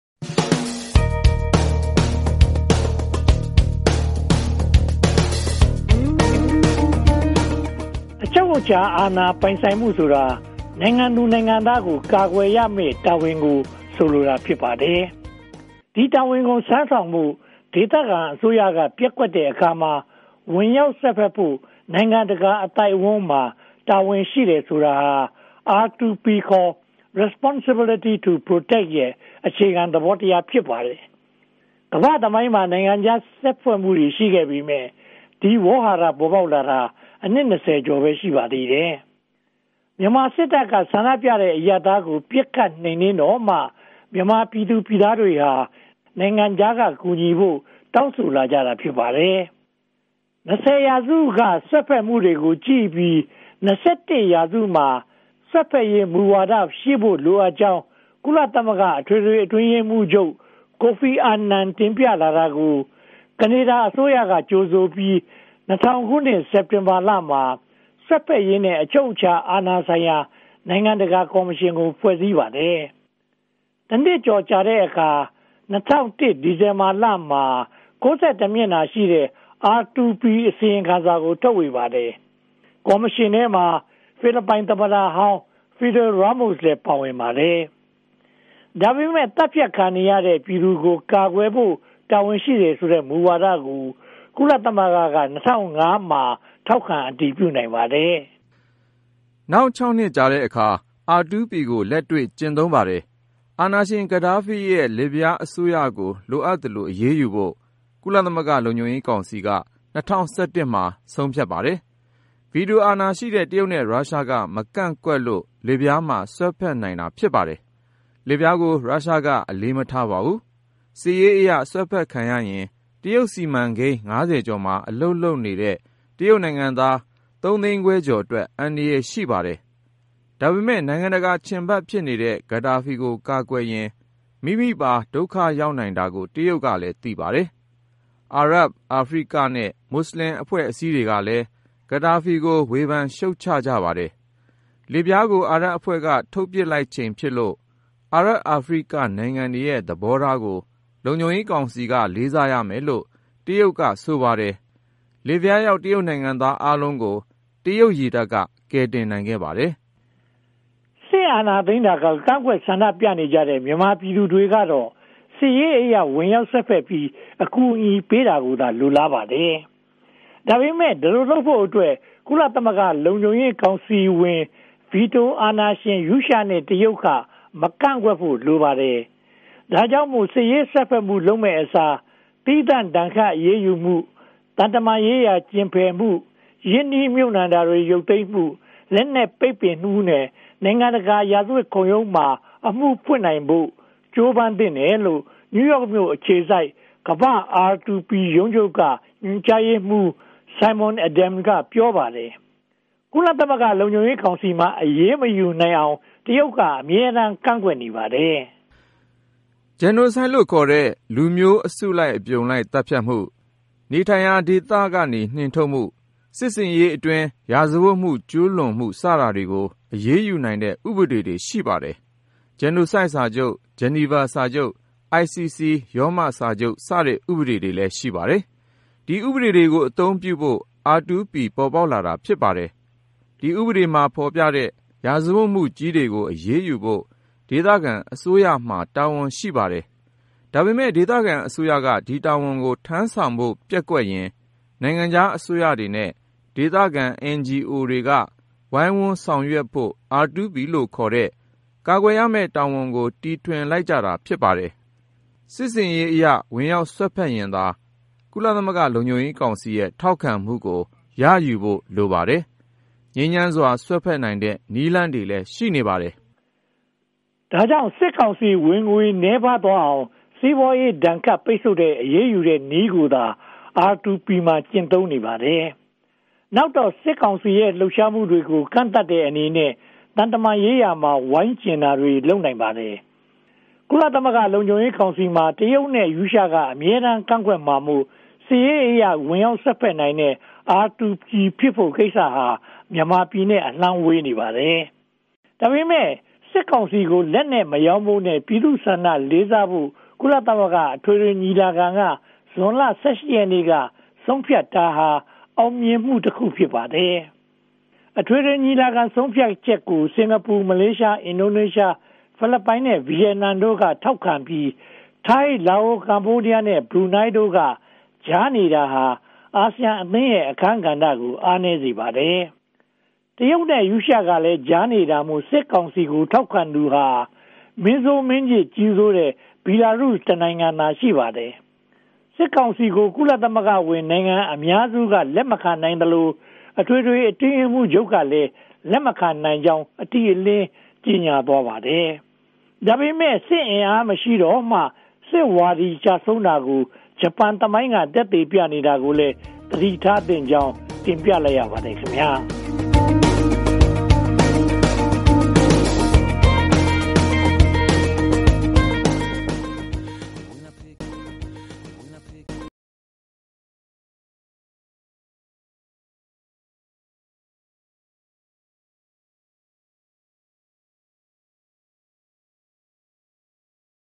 ကာကွယ်ရေးတာဝန် (R2P) ဆိုတာ ဘာလဲ
ကုလသမဂ္ဂလုံခြံရေးကောင်စီမှာ တရုတ်နဲ့ ရုရှားက အမြဲတမ်းကန့်ကွက်မှာမို့ စစ်ရေးအရ ဝင်ရောက်စွက်နိုင်တဲ့ R2P ဖြစ် ကိစ္စဟာ မြန်မာနဲ့ အလှမ်းဝေးနေပါတယ်။ ဒါပေမဲ့ စစ်ကောင်စီကို လက်နက်မရောင်းဖို့နဲ့ ပြည်သူ့ဆန္ဒကိုလေးစားဖို့ ကုလသမဂ္ဂအထွေထွေညီလာခံက ဇွန်လ (၁၈) ရက်နေ့က ဆုံးဖြတ်ထားတာဟ အောင်မြင်မှုတခု ဖြစ်ပါတယ်။

VOA (မြန်မာပိုင်း) စာမျက်နှာကို ဖတ်ရှုနိုင်တဲ့ links များကို ဝင်ရောက်ရန်
burme1.global.ssl.fastly.net
burmes1.azureedge.net
729411.info